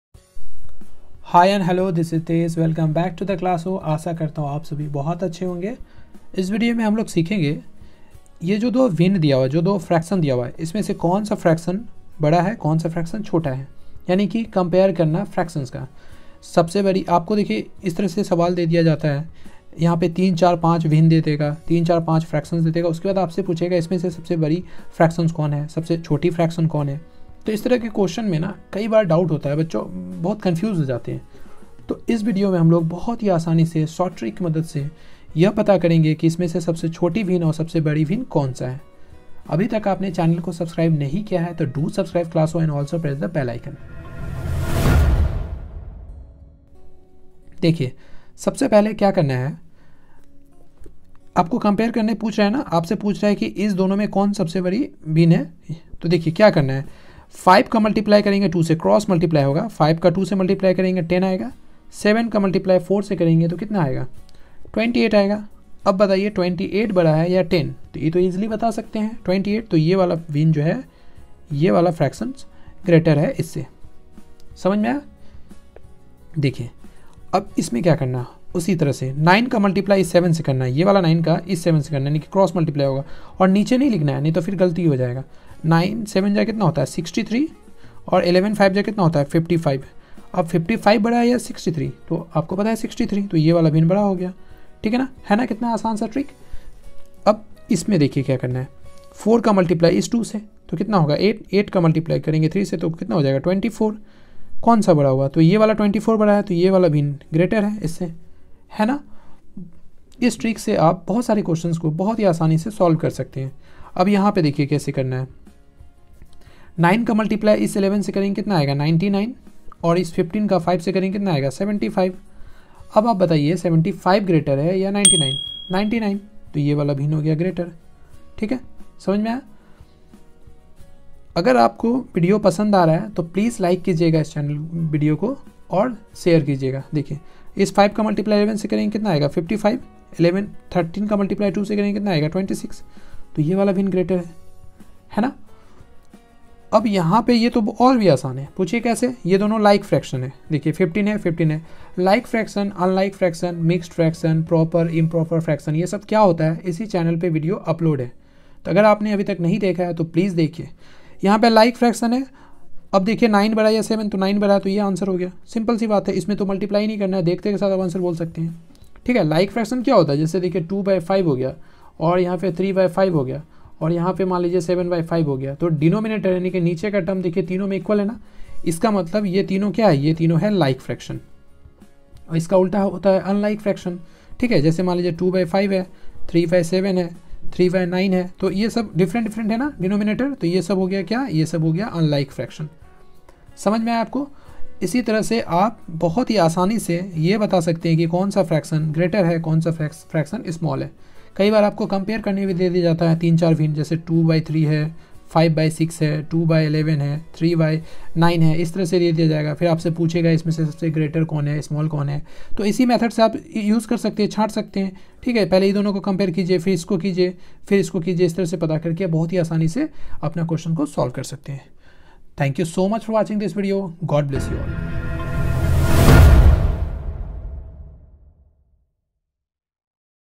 कौन सी भिन्न सबसे बड़ी और सबसे छोटी है । Comparison of Fractions in Hindi || Classo
Hi Everyone,
                    यह वीडियो में हमलोग सीखेंगे भिन्न को भिन्न से कैसे कंपैरिजन करते है शॉर्ट ट्रिक्स के मदद से, इस तरह के सवाल आपके एग्जाम में पूछे जाते है ।

बिहार सामान्य ज्ञान ||Bihar Samanya Gyan|| बिहार से जुड़े कुछ महत्वपूर्ण सवालों के जवाब for All Exam

          Team "Classo"